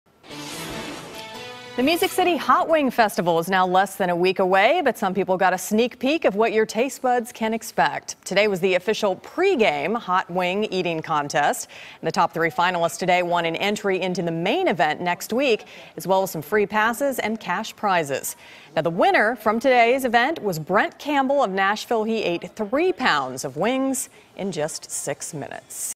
Eating Contest Held Ahead Of Hot Wing Festival
The Music City Hot Wing Festival has been quickly approaching, but some people got a sneak peek of what taste buds can expect.